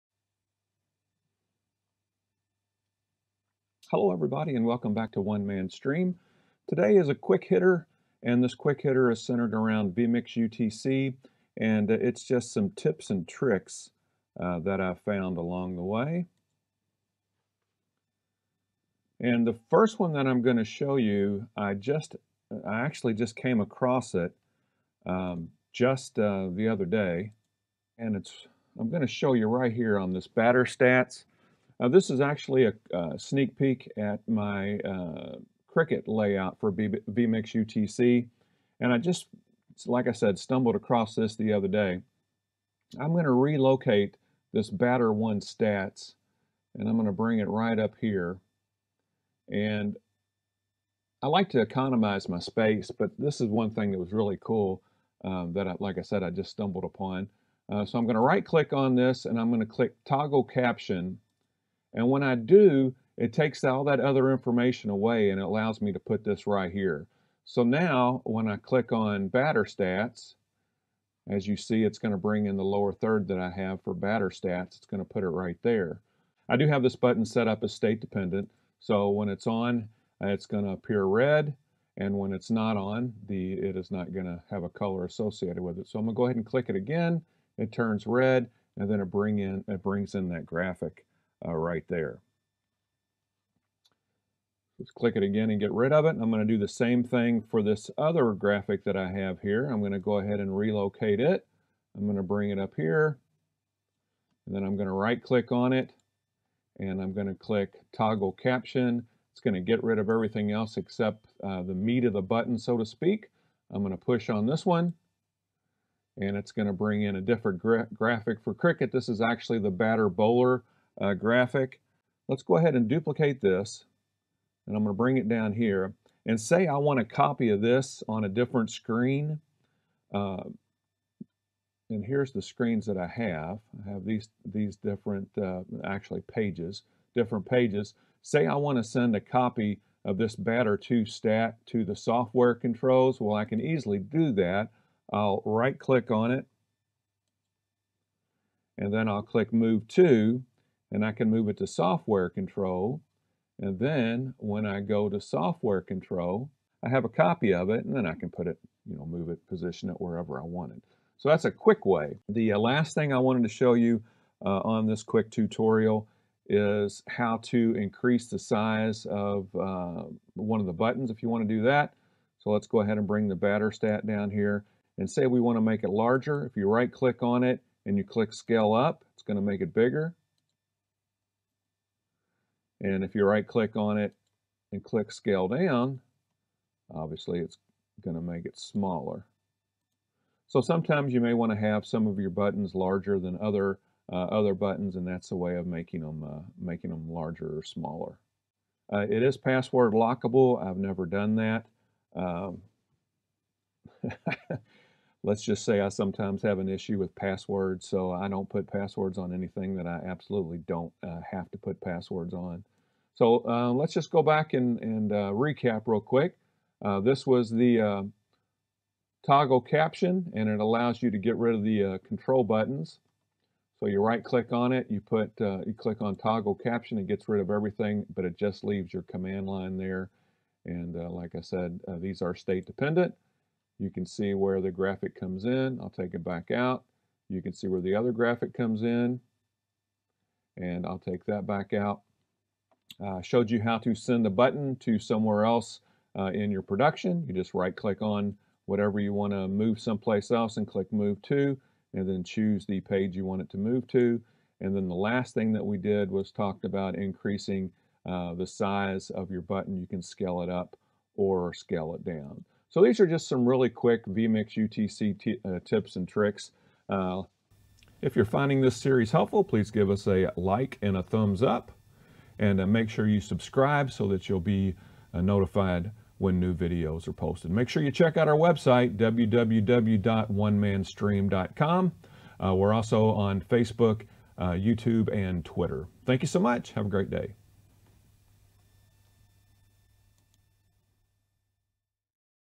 vMix UTC Tips & Tricks | One Man's Stream Episode 13 | vMix UTC Tutorial
In this episode of One Man's Stream I demonstrate a few vMix UTC Tips & Tricks which I have uncovered along the way.  Hard to believe that this is already the 18th episode in this tutorial series.  Thanks to all of you for the comments, suggestions and support.

vMix UTC is a FREE companion software for vMix and be downloaded at the above link.